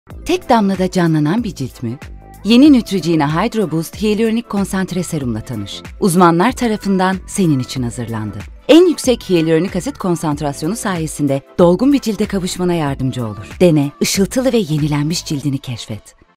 Yeni Neutrogena Hydro Boost Hiyalüronik Konsantre Serum’u hemen keşfet!
Neutrogena Hydro Boost Hiyalüronik Konsantre Serum ile tek damlada canlanan bir cilt mümkün.💙  Üstelik en yüksek hiyalüronik asit konsantrasyonu* sayesinde dolgun bir cilde kavuşmana yardımcı olur.
Işıltılı ve yenilenmiş cildini hemen sen de keşfet!

*Neutrogena Hydro Boost ürünleri arasında en yüksek oran.